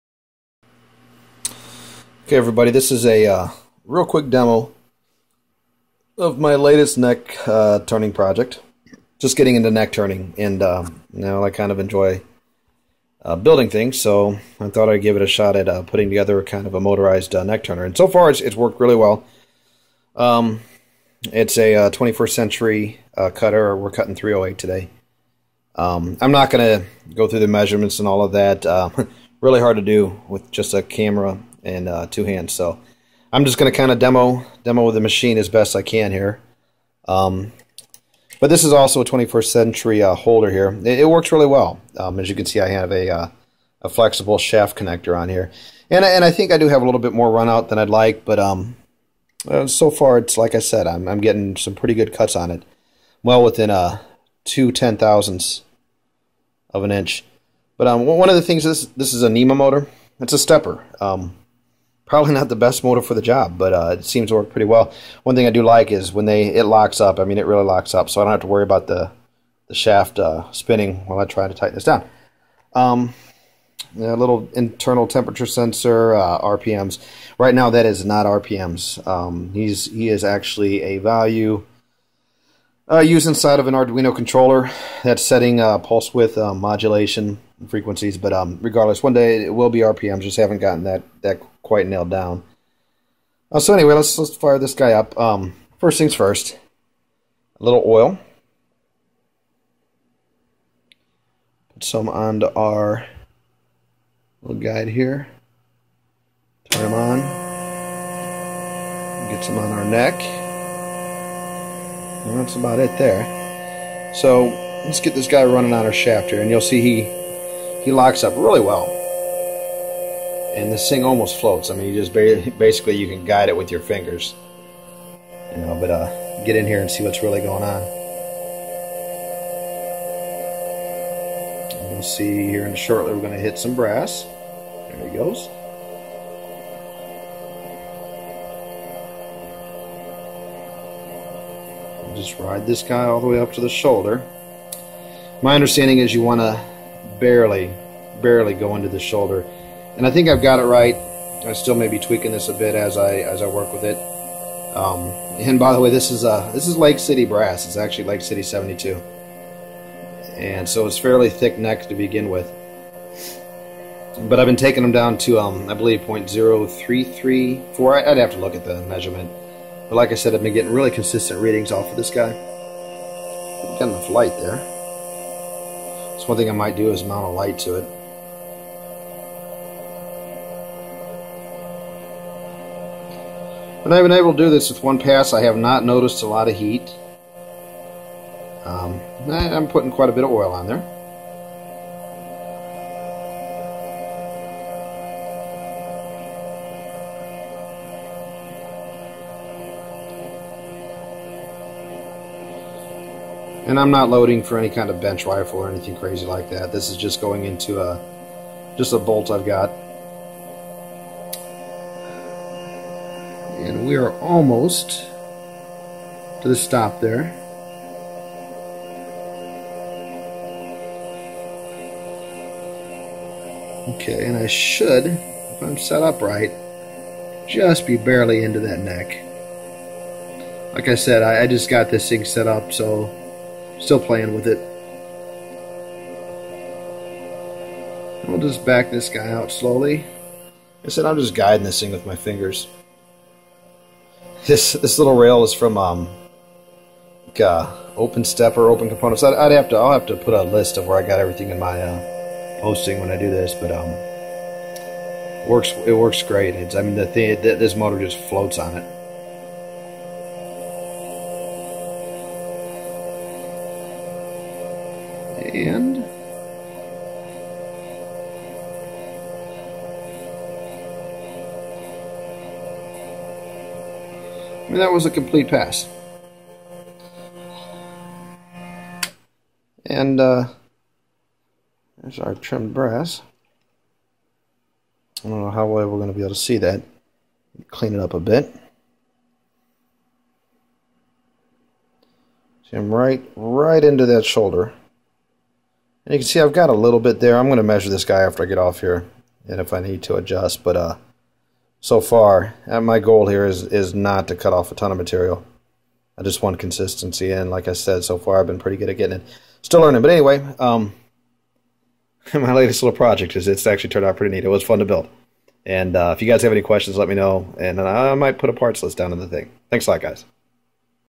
Motorized neck turner
A DIY Motorized Neck Turner using a 21st Century Neck Turner, Nema 24 Stepper Motor, and Arduino